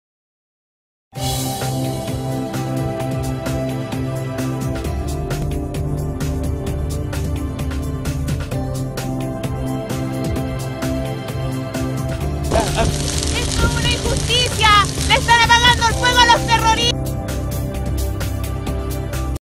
Un bombero lesionado y un carro quemado en incendio de predios agrícolas en Ercilla
Según informaciones preliminares, los terrenos corresponderían a la Familia Urban.